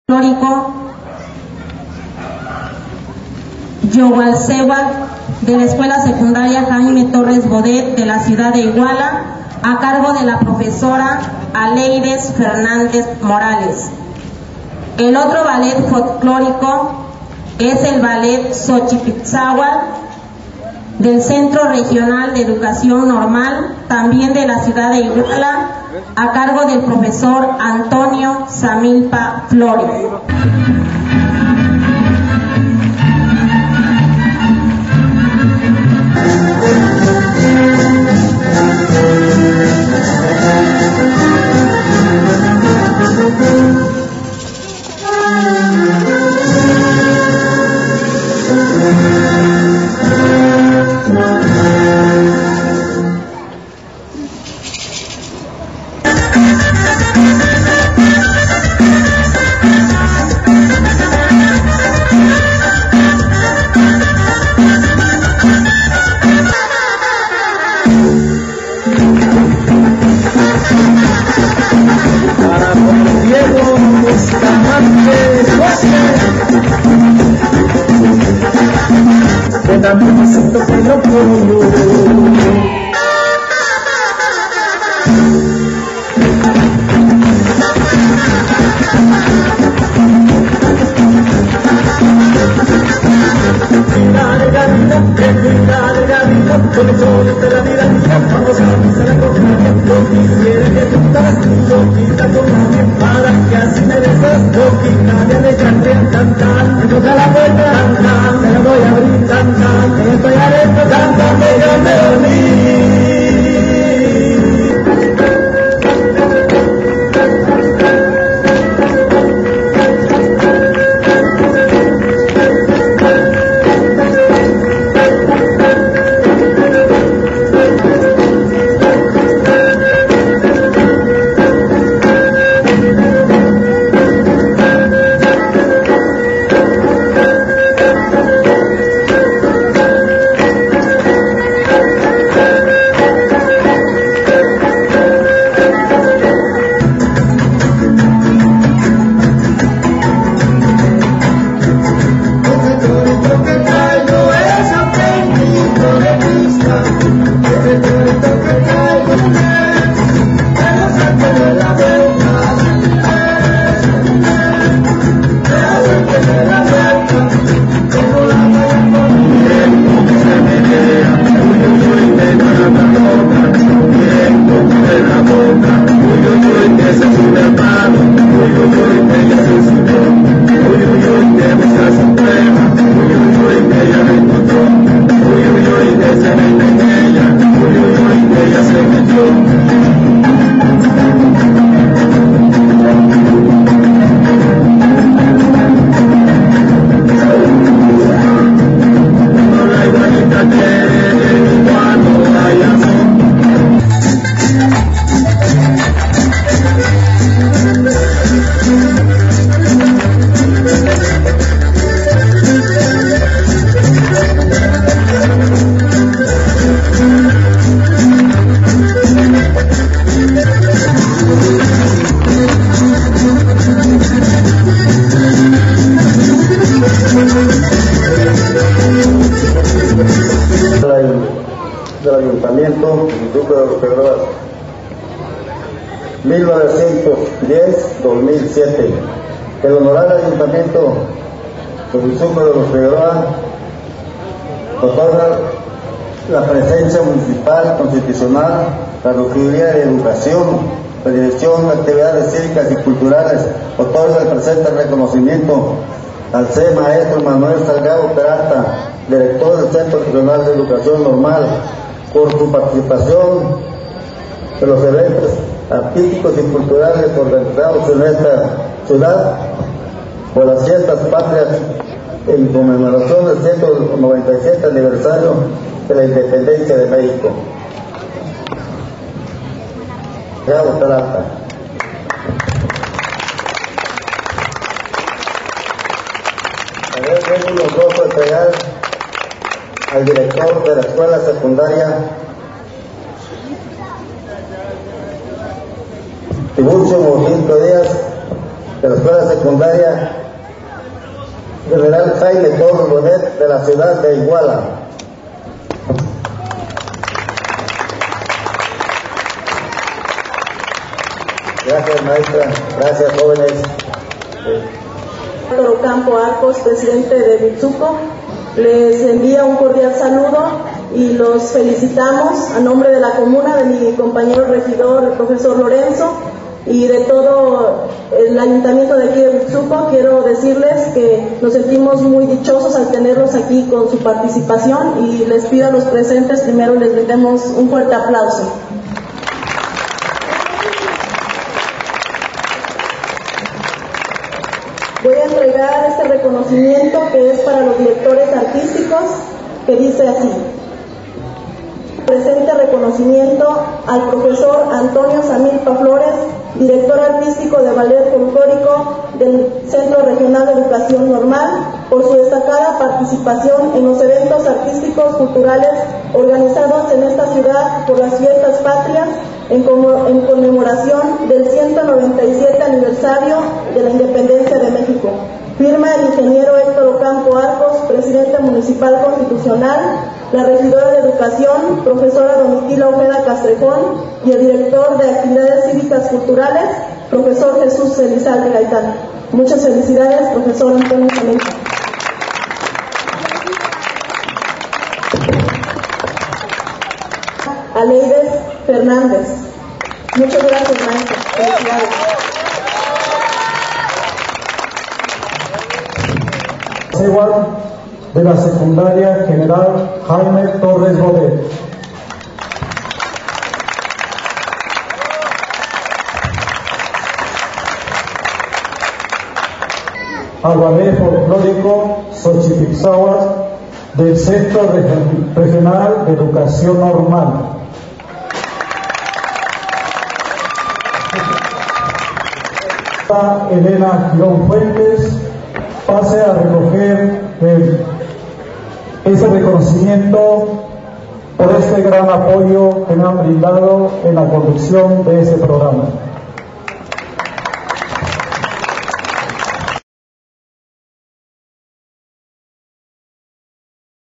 Espacio Cultural Huitzuco Fiestas Patrias 2007
Huitzuco, Gro. Ballet folklorico Yohualcehuatl y Xochipitzahuatl dirigidos por Tiburcio Mojica Diaz y Manuel Salgado Peralta. Iguala de la Independencia, Guerrero. huitzuco.com